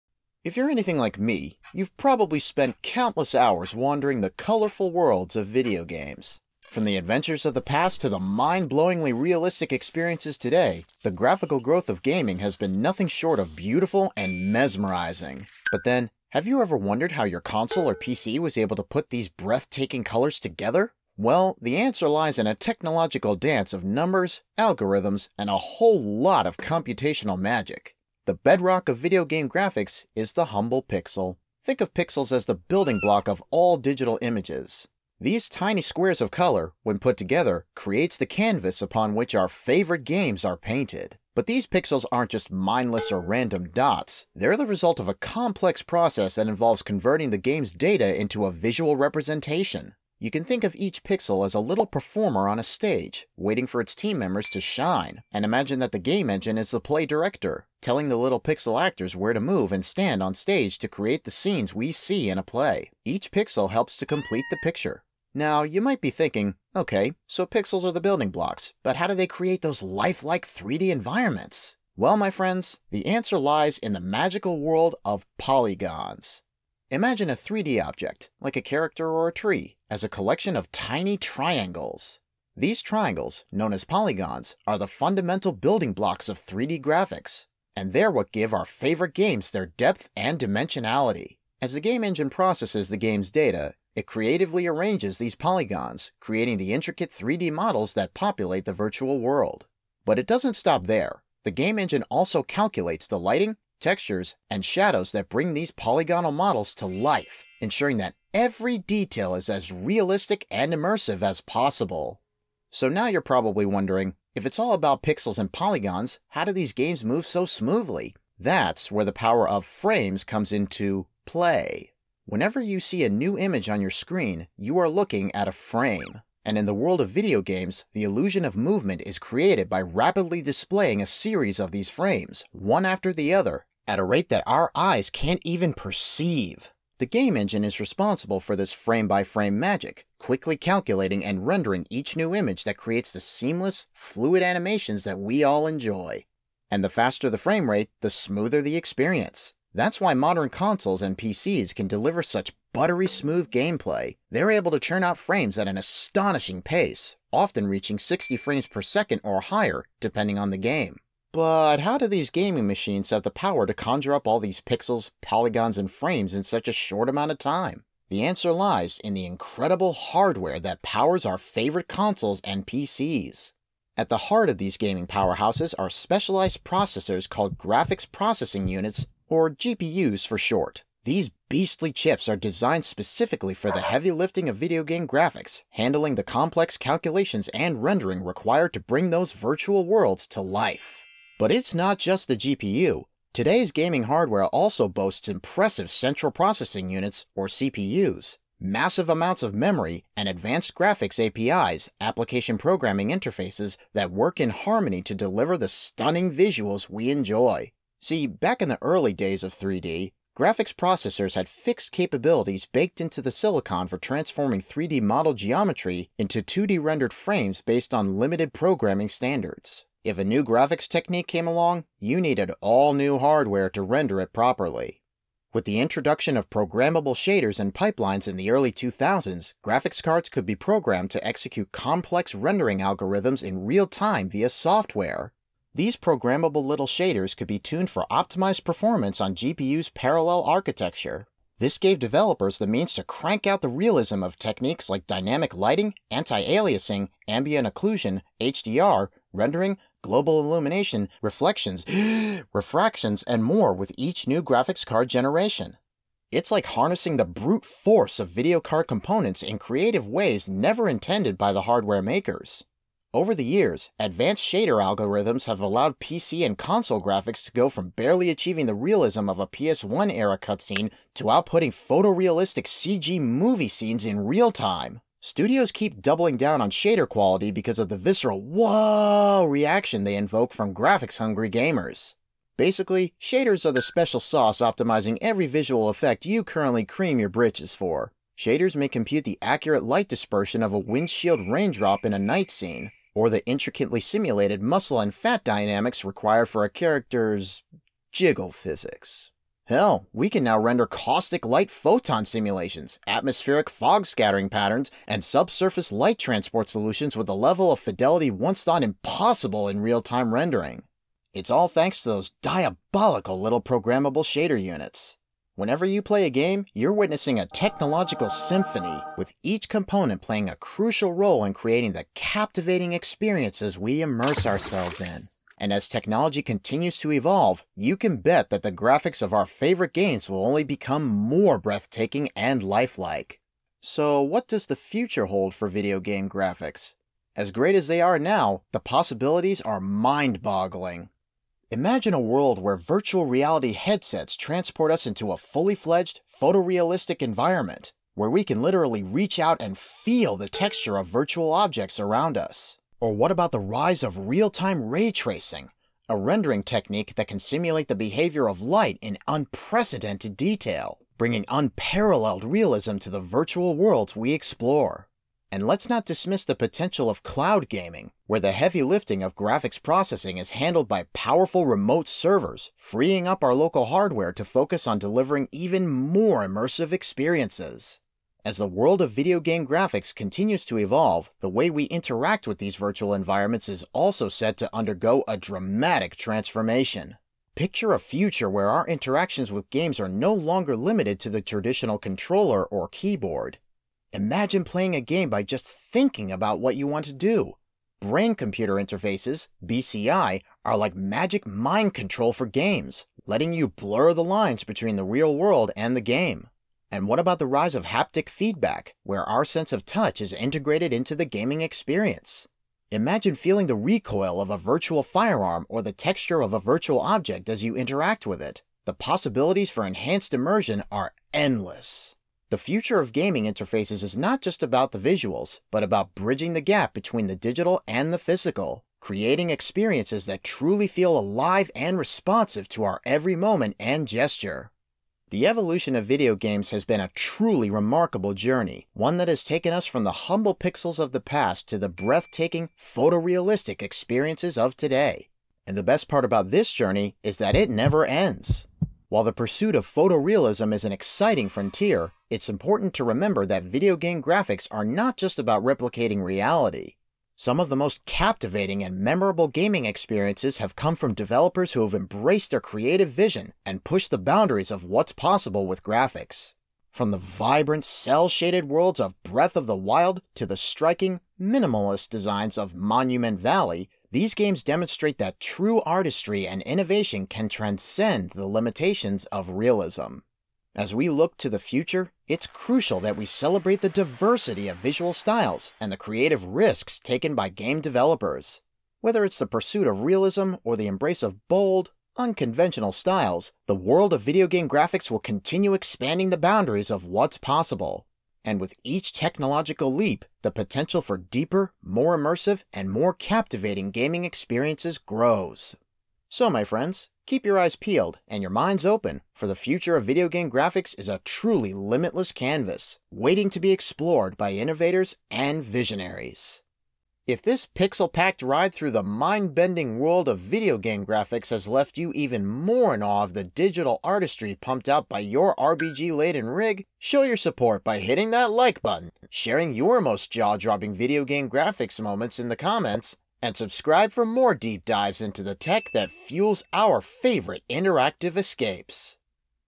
How Do Video Game Graphics ACTUALLY Work?
How Do Video Game Graphics Work???
🎮 Dive into the fascinating world of video game graphics! From pixels to polygons, discover the magic behind your favorite games.
🕒 Timestamps:
0:00 - Introduction
00:33 - The Power of Pixels
1:16 - Polygons: Building 3D Worlds
2:06 - Frames and Smooth Animation
2:54 - Gaming Hardware: GPUs and More
3:46 - Programmable Shaders Revolution
5:35 - The Future of Gaming Graphics
8:30 - Conclusion

Introduction
Embark on a journey through the evolution of video game graphics, from classic pixelated adventures to today's mind-blowing realistic experiences.
The Power of Pixels
Explore the fundamental building blocks of digital images. Learn how these tiny squares of color come together to create the vibrant worlds we love to explore.
Polygons: Building 3D Worlds
Discover how triangular polygons form the backbone of 3D graphics, bringing depth and dimension to virtual environments.
Frames and Smooth Animation
Uncover the secret behind fluid gameplay as we explain how rapid frame rendering creates the illusion of seamless movement.
Gaming Hardware: GPUs and More
Peek under the hood of modern gaming machines and learn about the powerful components that make stunning graphics possible.
Programmable Shaders Revolution
Dive into the game-changing world of programmable shaders and how they've transformed the landscape of real-time rendering.
The Future of Gaming Graphics
Get a glimpse of what's to come, from photorealistic VR experiences to brain-computer interfaces that could revolutionize how we interact with games.

🌱 Main Takeaways:

Video game graphics have evolved from simple pixels to complex 3D environments
Polygons, frames, and specialized hardware work together to create immersive experiences
Programmable shaders have dramatically improved graphics capabilities
The future of gaming graphics holds exciting possibilities beyond photorealism

💬 What's your favorite era of video game graphics? Share in the comments below!